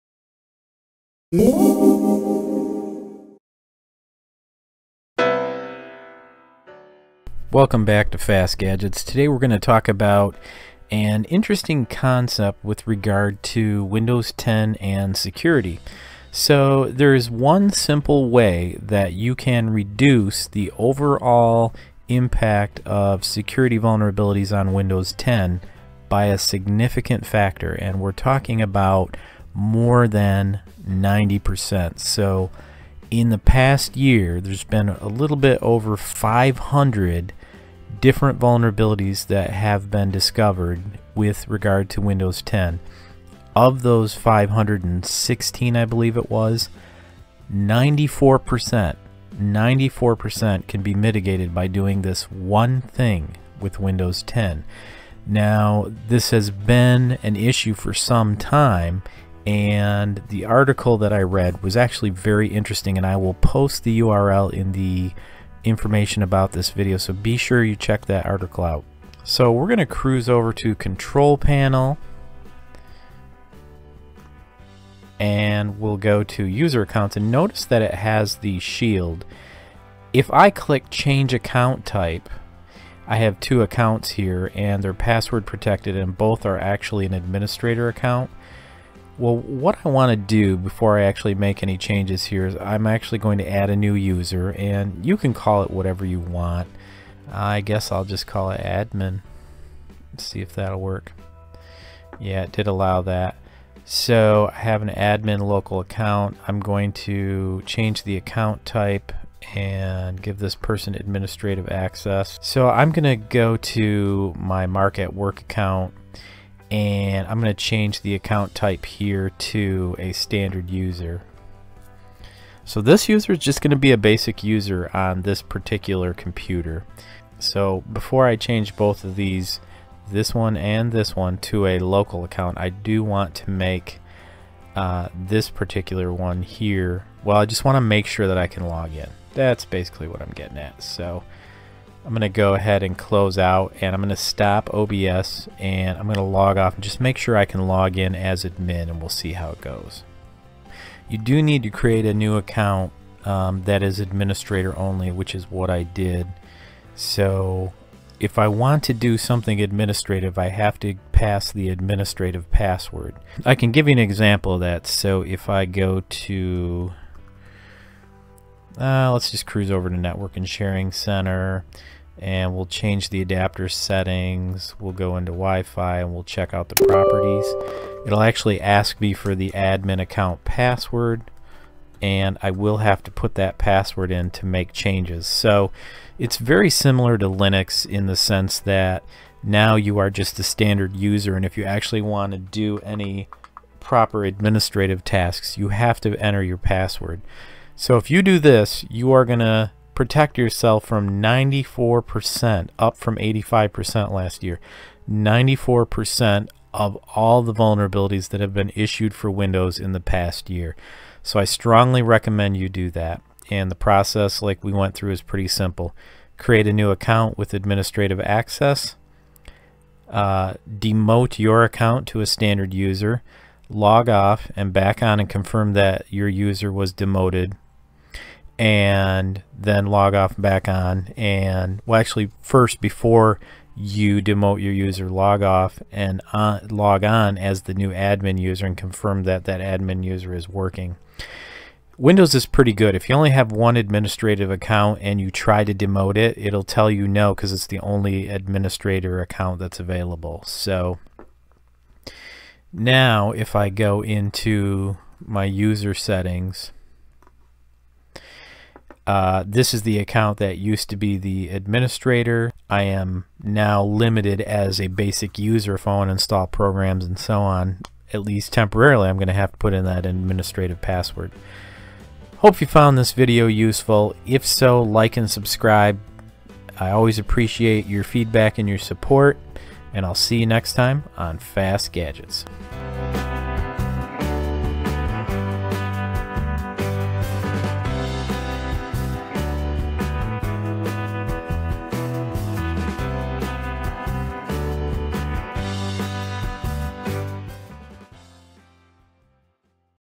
Do this, Block 94% of Windows Vulnerabilities
Is it possible to block the majority of Windows vulnerabilities with a relatively simple system change?  Don't miss this video to find out!